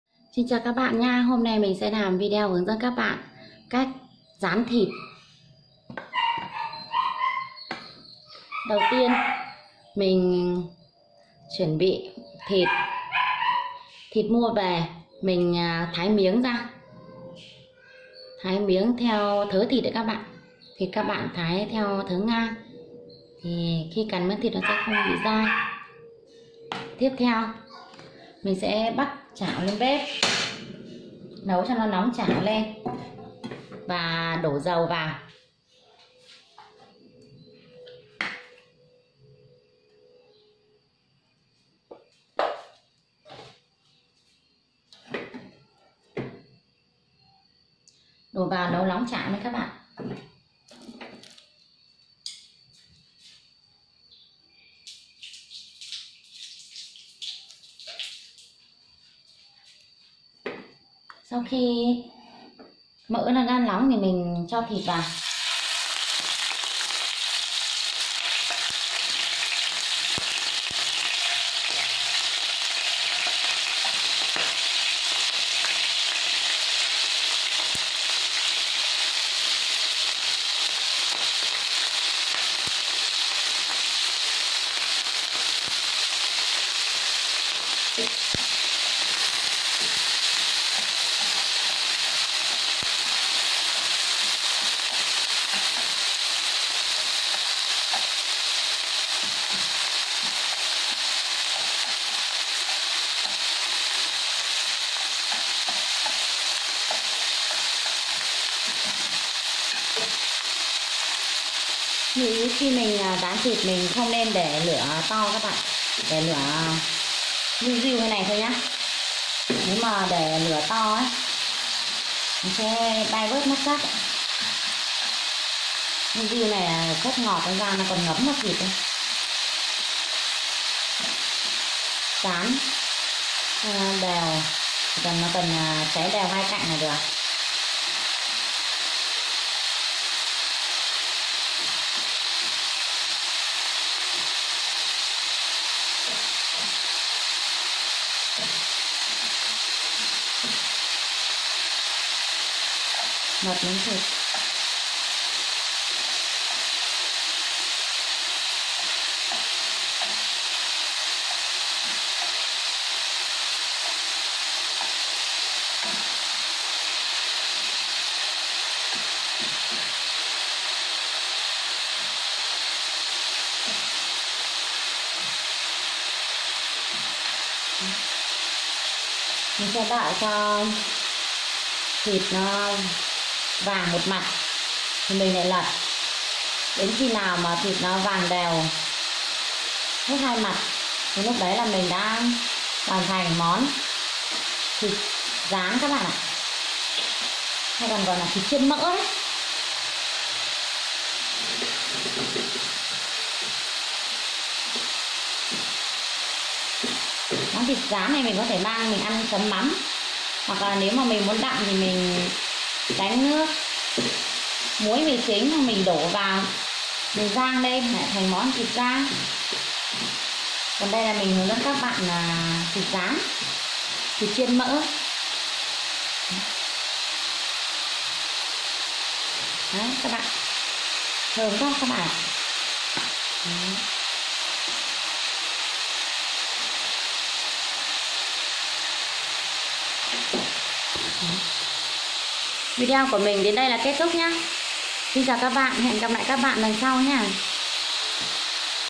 spencer hamilton // Cách chiên thịt chuẩn vị thơm ngon của chị gái
Cách chiên thịt chuẩn vị thơm ngon của chị gái
Chị gái đảm đang dạy cách chiên thịt đúng cách